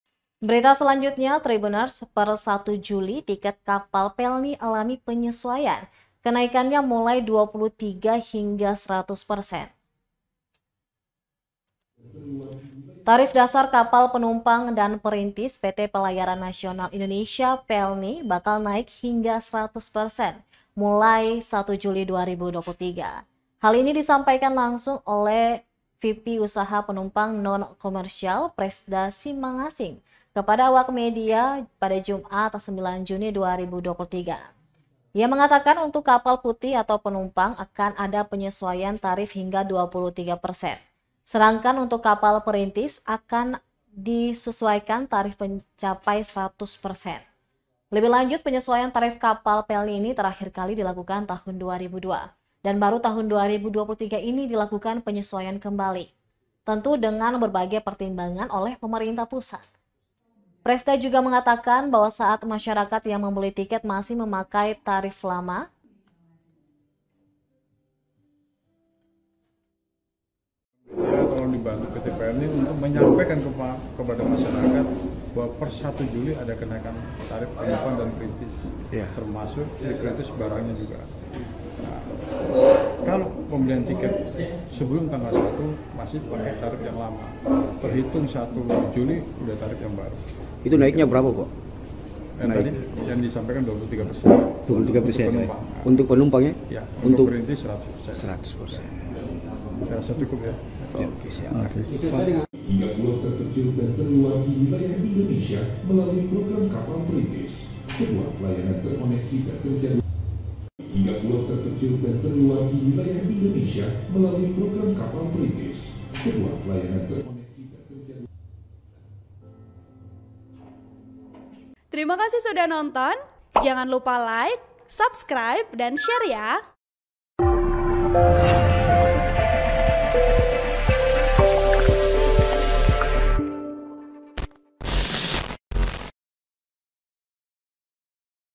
Per 1 Juli Tiket Kapal Pelni Alami Penyusuaian: Kenaikan Mulai 23 Hingga 100 Persen
ED, Laporan Wartawan TribunAmbon.com, Alfin Risanto

AMBON, TRIBUNAMBON.COM – Tarif dasar kapal penumpang dan perintis PT Pelayaran Nasional Indonesia (Pelni) bakal naik hingga 100 persen mulai per 1 Juli 2023.

Hal ini disampaikan langsung oleh VP usaha penumpang Non Komersial Presda Simangasing kepada awak media, Jumat (9/6/2023).

Dikatakan, untuk kapal Putih atau penumpang akan ada penyesuaian tarif hingga 23 persen.

Sedangan untuk kapal perintis akan di penyusuaian tarif mencapai 100 persen.

Lebih lanjut, penyesuaian tarif kapal Pelni ini terkahir kali dilakukan tahun 2002.

Dan baru tahun 2023 ini dilakukan penyusuaian kembali, tentu dengan berbagai pertimbangan oleh pemerintah Pusat.

Presda juga mengatakan bahwa saat masyarakat yang membeli tiket masih memakai tarif lama.